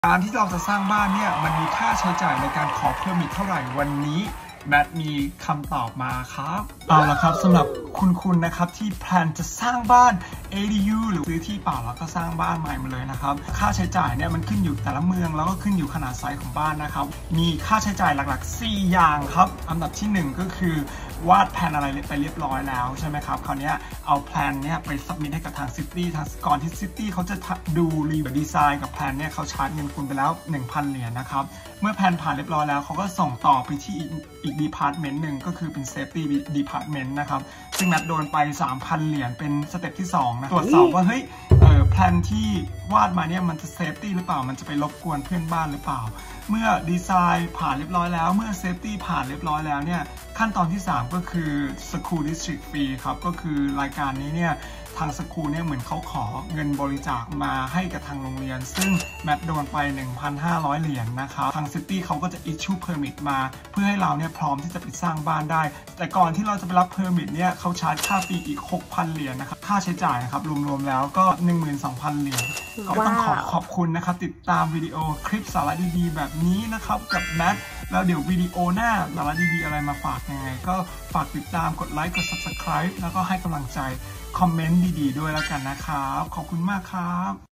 ค่าใช้จ่าย การ ขอ Permit สร้าง บ้าน ในอเมริกา
เอาละครับวีดีโอนี้แมท มาแจกแจงค่าใช้จ่ายในการขอ Permit ในการสร้างบ้านในอเมริกาเป็นความรู้กันนะครับ ทั้งนี้ทั้งนั้นค่าใช้จ่าย ขึ้นอยุ่กับขนาดและเมืองต่างๆ นะครับ วีดีโอนี้เป็นแค่ Idea ของคนที่กำลัง converts Garage เป็น บ้านหลังน้อย ขนาด 600 SQFT ครับ
ซื้อขายลงทุน บ้าน ในแอลเอ นึกถึง realtor Matt ครับ #02081898